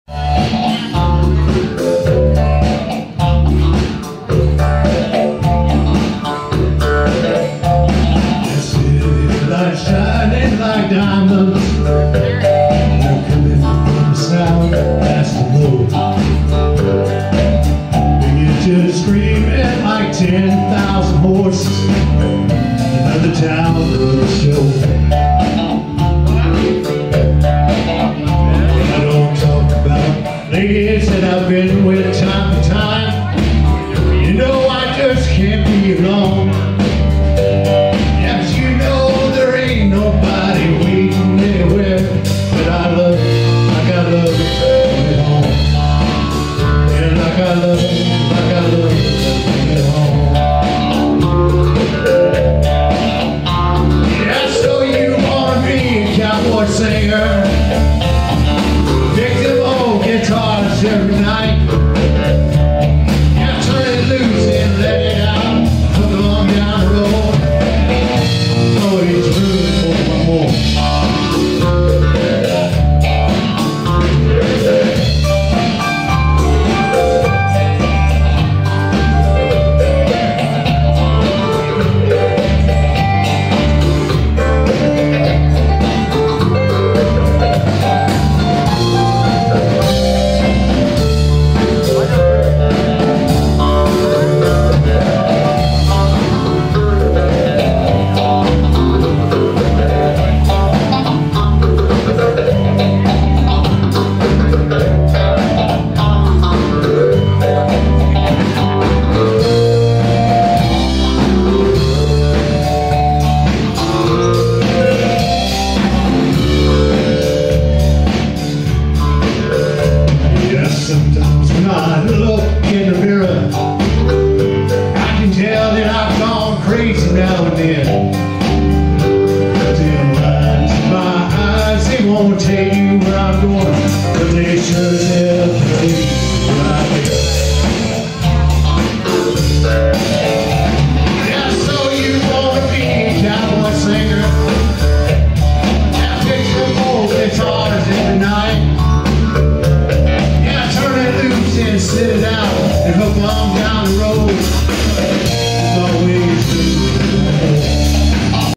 Terry Greene Live in Las Vegas
Terry Greene live with Chris Heers and the Dirt Rich Band  at Arizona Charlie's Decatur on Las Vegas ..  October 31, 2014.. If you're ever in Vegas definitely a Band you gotta see.. They are amazing